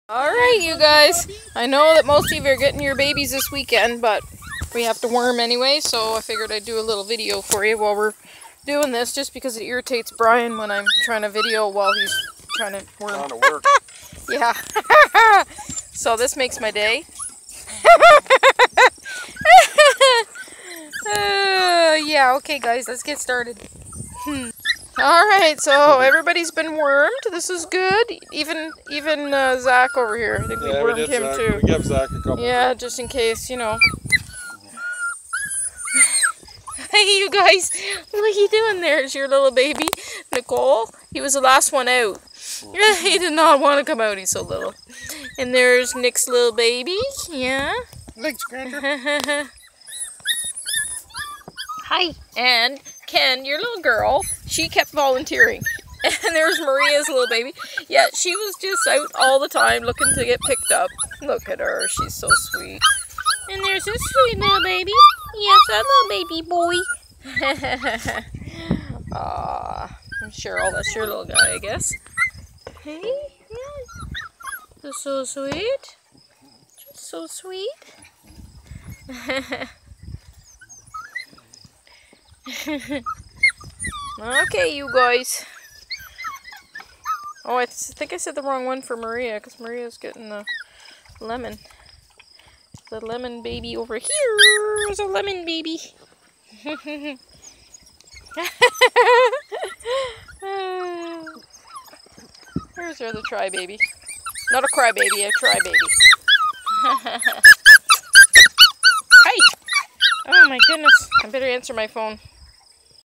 Beagle bonanza! Rippin' & tearin'
Rensies little pups are heading home this weekend!